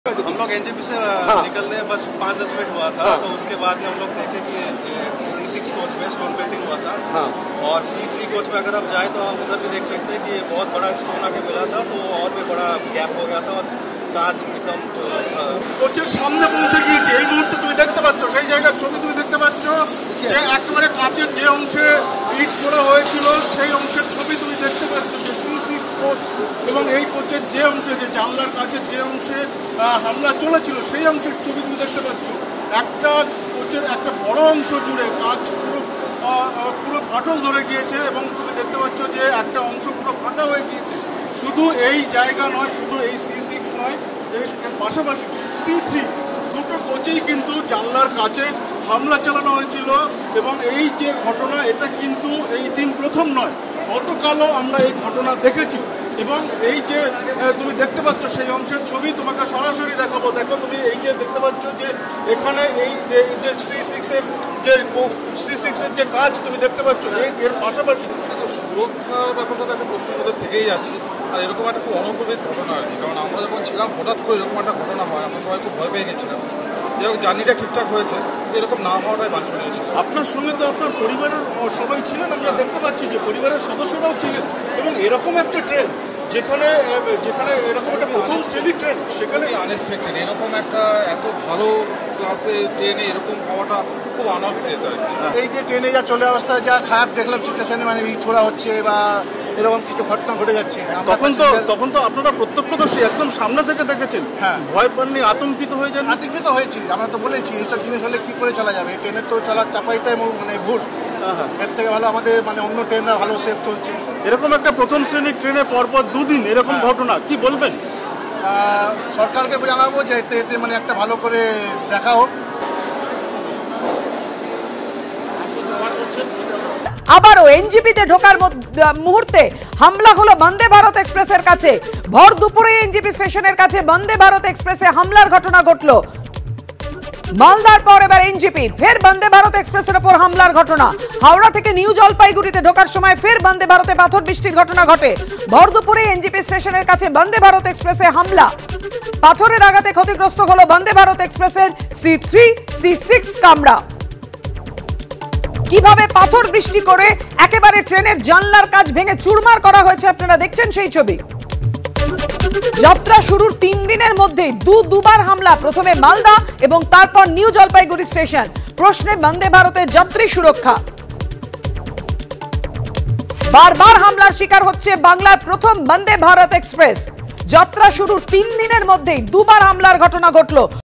Vande Bharat Express: কোথায় নিরাপত্তা? প্রশ্ন বন্দে ভারতের যাত্রীদের
মালদার পরে এবার এনজেপি, ফের বন্দে ভারত এক্সপ্রেসের উপর হামলা
হাওড়া থেকে এনজেপি ঢোকার সময় ফের বন্দে ভারতে পাথরবৃষ্টি
ভর দুপুরেই এনজেপি স্টেশনের কাছে বন্দে ভারত এক্সপ্রেসে হামলা ---
পাথরের আঘাতে ক্ষতিগ্রস্ত বন্দে ভারত এক্সপ্রেসের সি-৩, সি-৬ কামরা
যাত্রা শুরুর ৩দিনের মধ্যে ২বার হামলা, প্রশ্নে বন্দে ভারতের যাত্রী সুরক্ষা
বারবার হামলার শিকার বাংলার প্রথম বন্দে ভারত এক্সপ্রেস